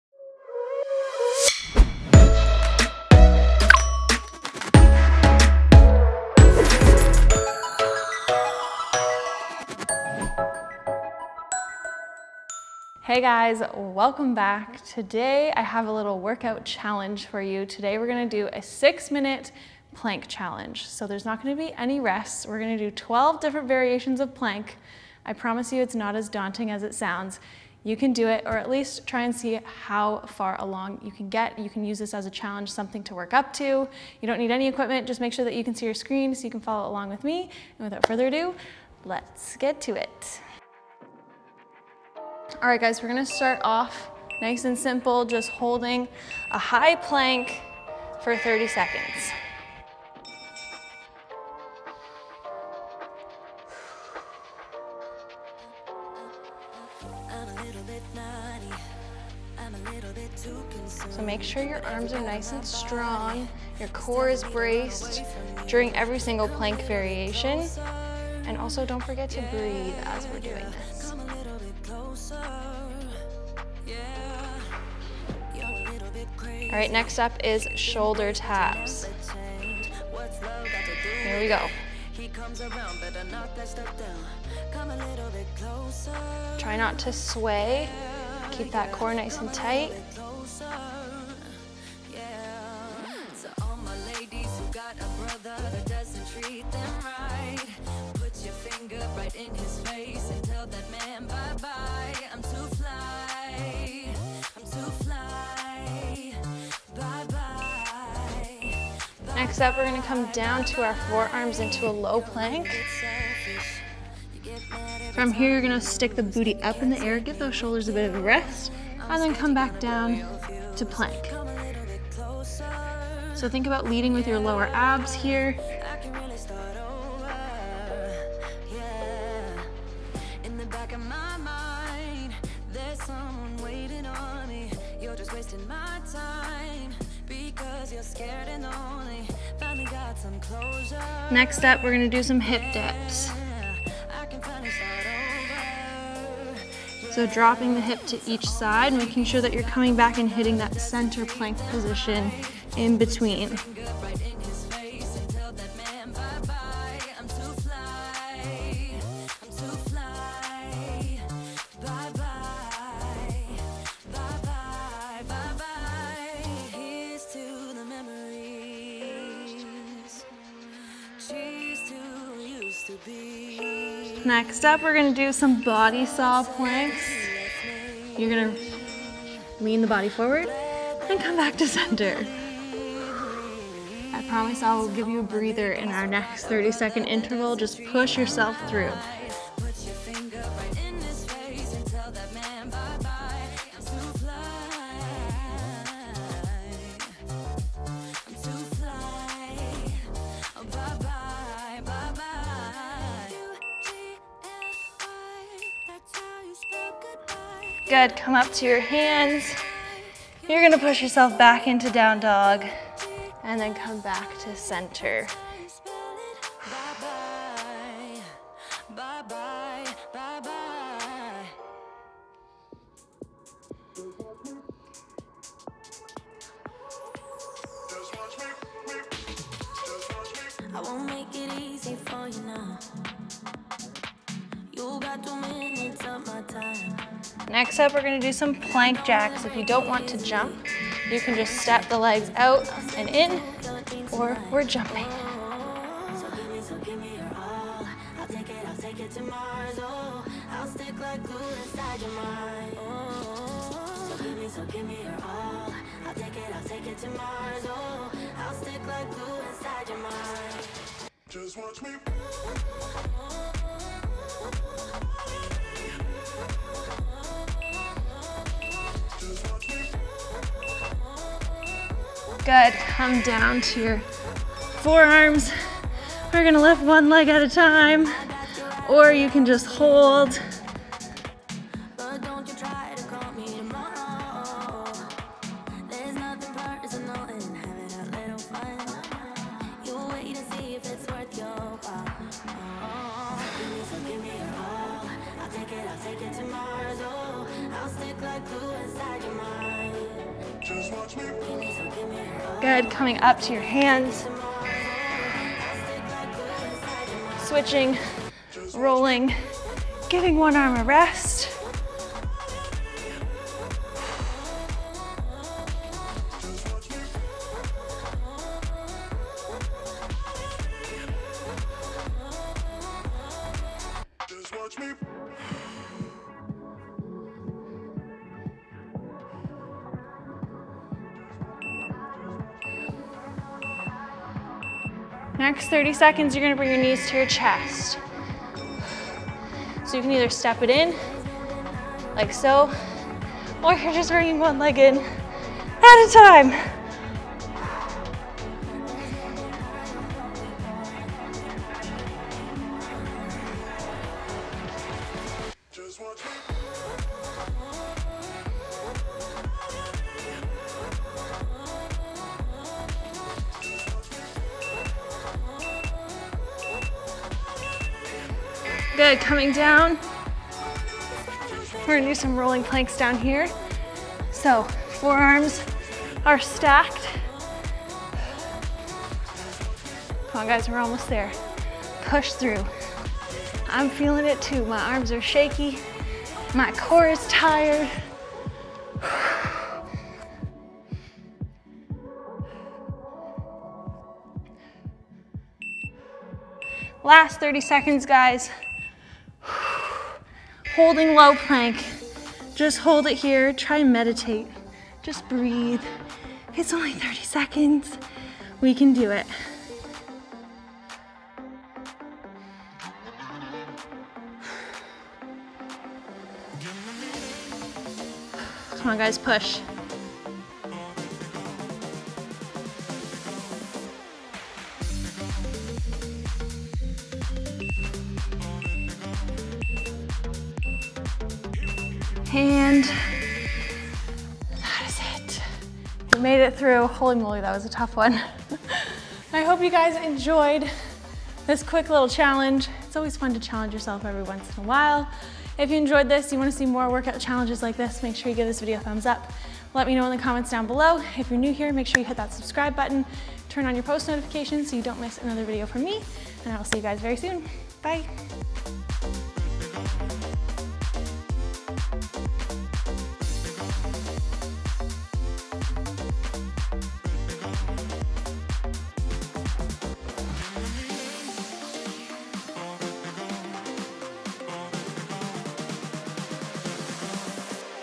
6 MIN PLANK CHALLENGE - A Core and Arm Workout!
Can you complete the challenge? Follow along with me as we do 12 different variations of plank in this quick and intense challenge. Your core and arms will be on fire by the end! 🔥

⭐️ DO THIS COOL DOWN AFTER (5 min): shorturl.at/jswT3

✘ I N S T A G R A M: @madfit.ig
✘ T W I T T E R: @maddielymburner
✘ F A C E B O O K